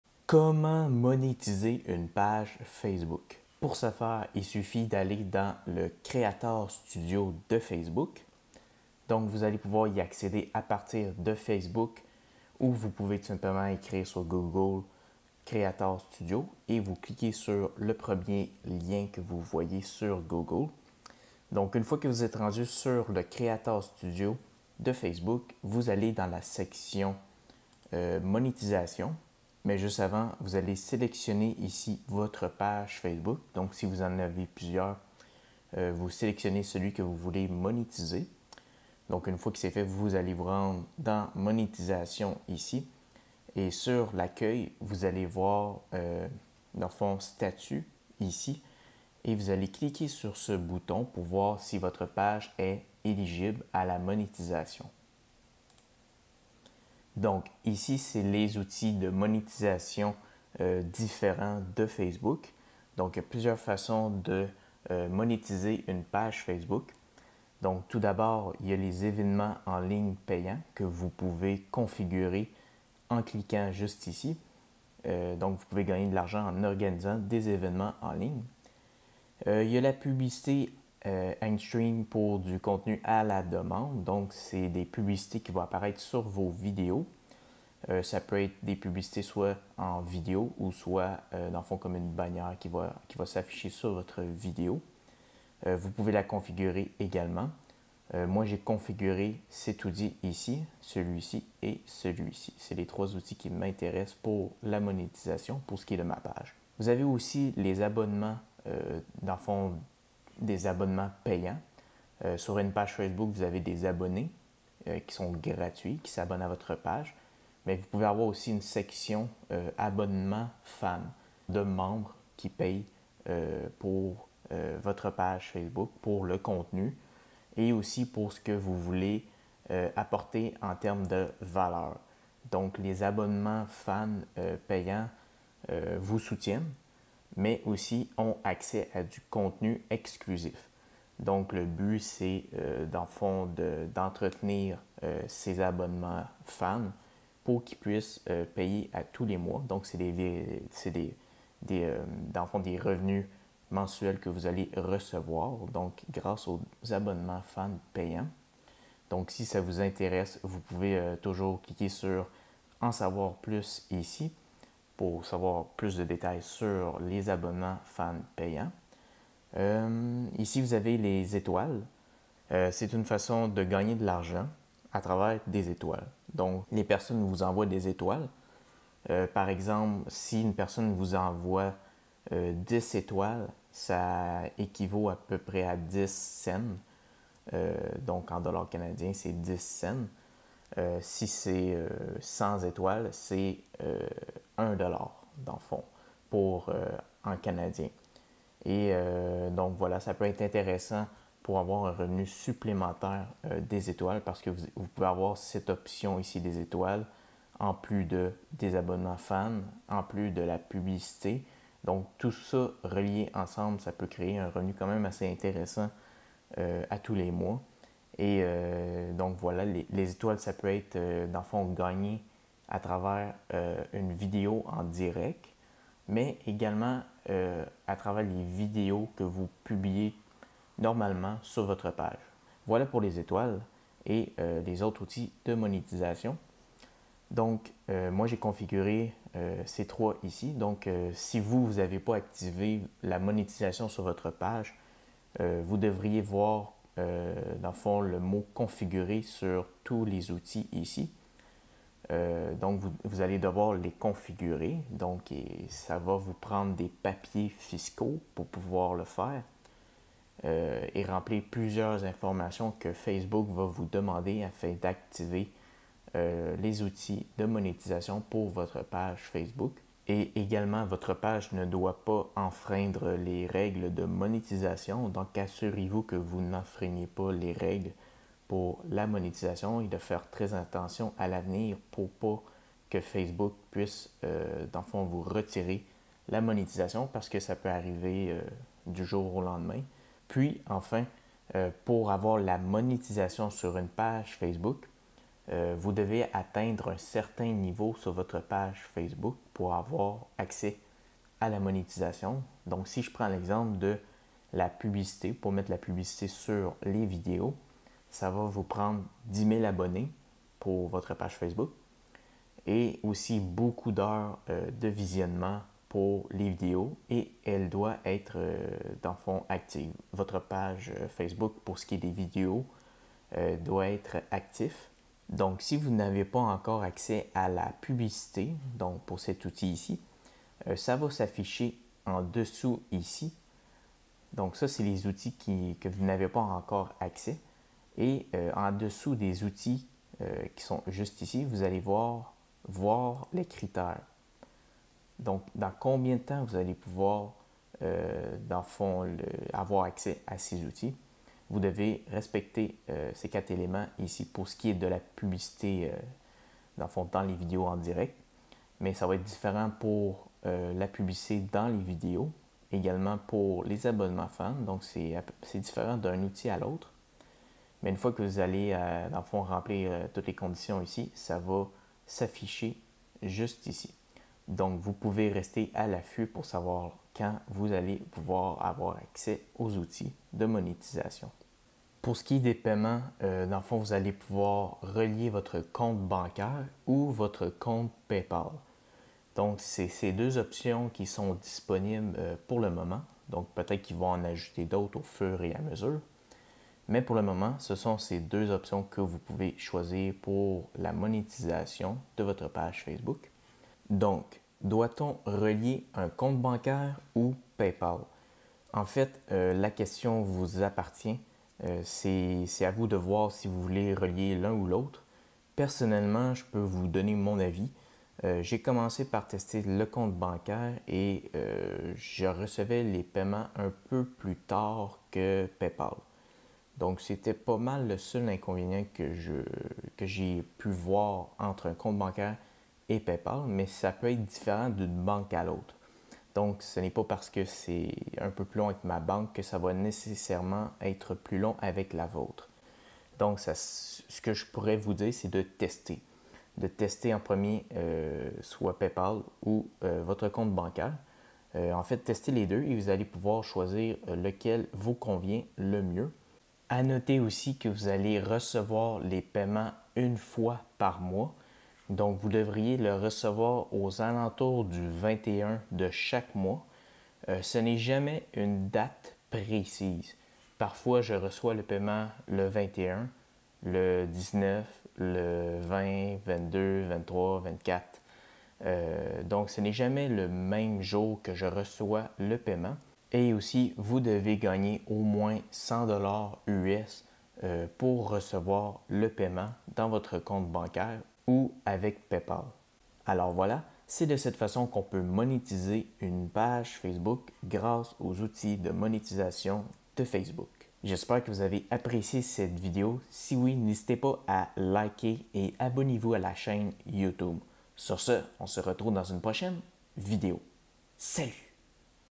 Comment monétiser sa page Facebook
Comment monétiser les vidéos Facebook ? Comment Facebook paye les vues ? Comment gagner de l'argent sur sa page Facebook ? Découvrez tout ce qu'il faut savoir pour monétiser une page Facebook.